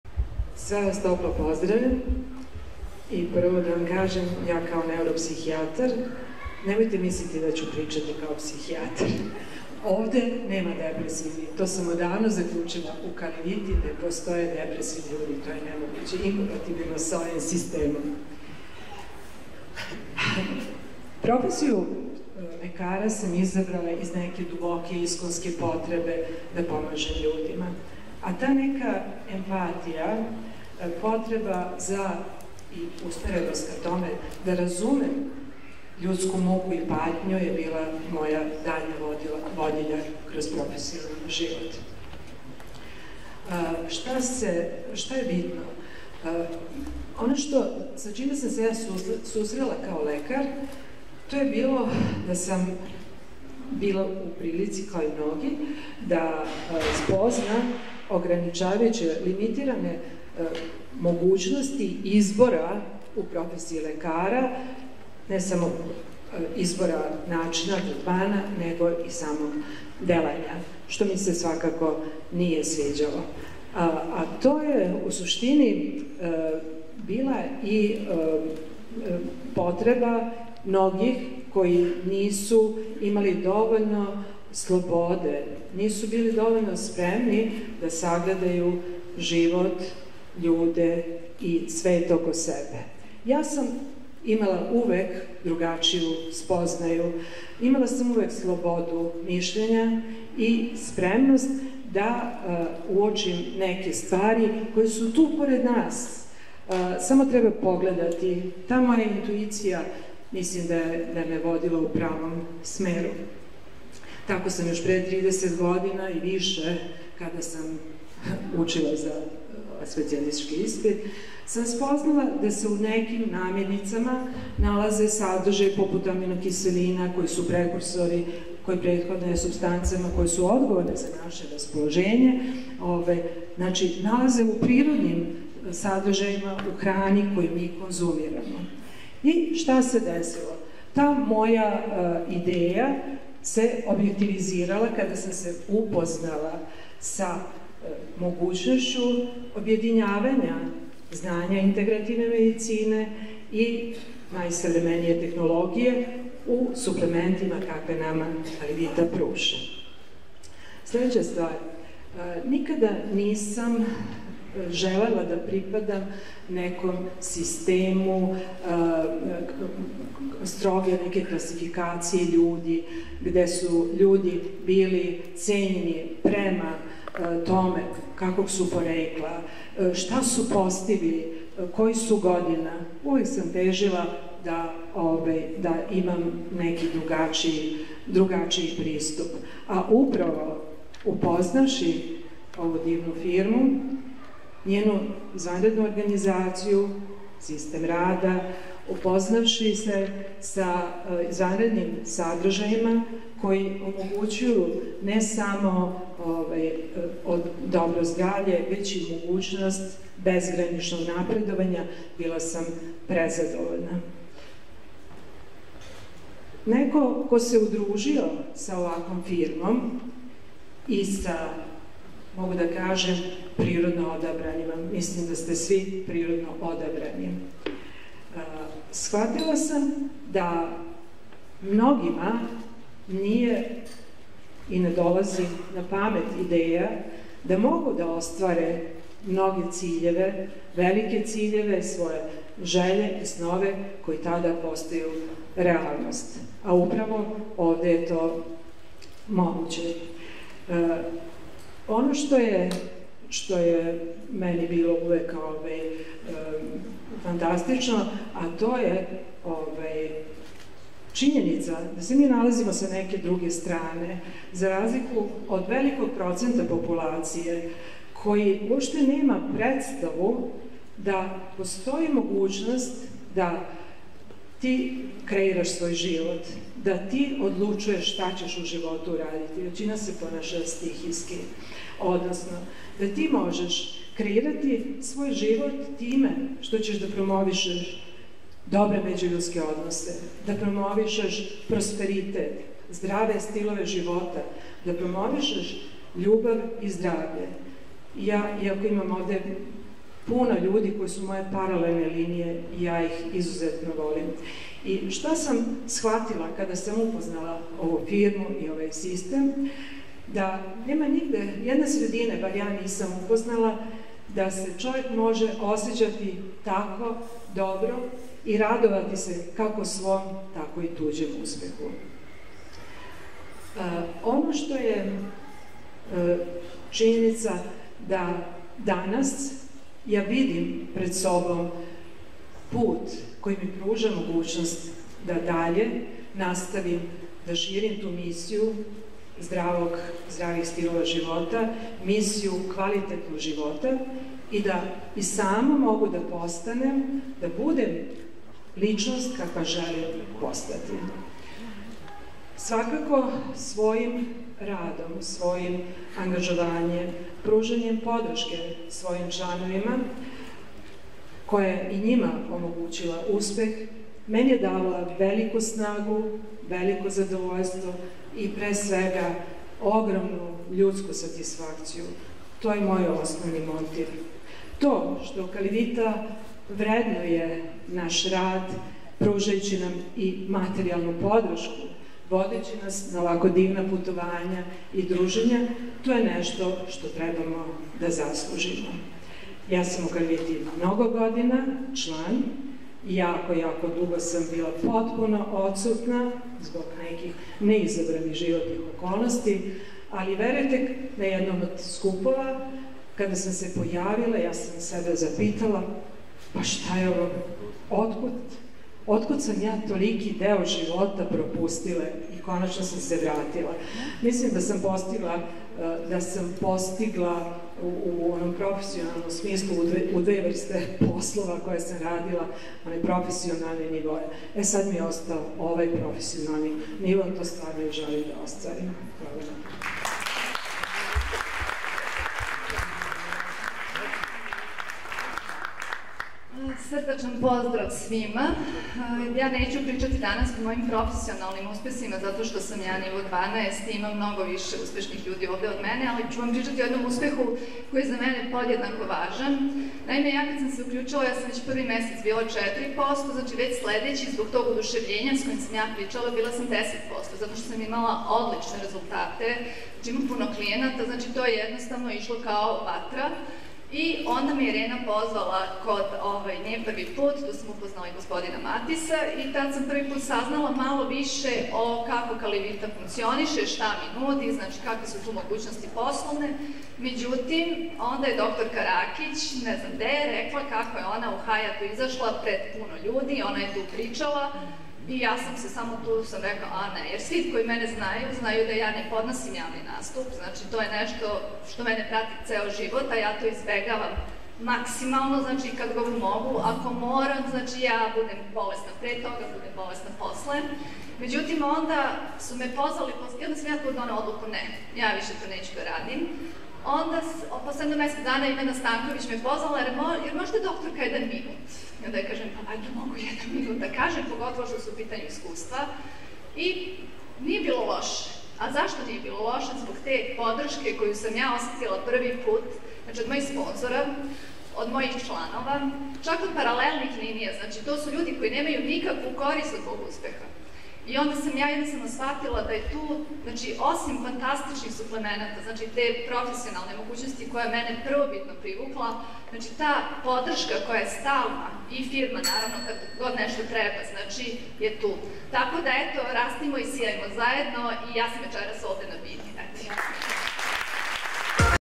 prim dr Vesna Bućan - Kako je biti ostvareni lekar u biznisu duplikacije?
prim dr Vesna Bućan (18%) - Kako je biti ostvareni lekar u biznisu duplikacije?
+ dr Irena Motika (12%)